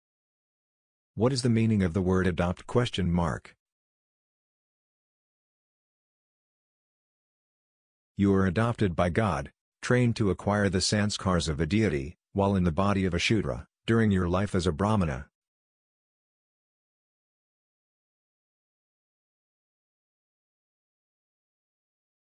‘ adopt ’ meaning
What is the meaning of the word ‘ adopt ’?

you are adopted by God :trained to acquire the sanskaras of a deity, while in the body of a shudra—during your life as a brahmana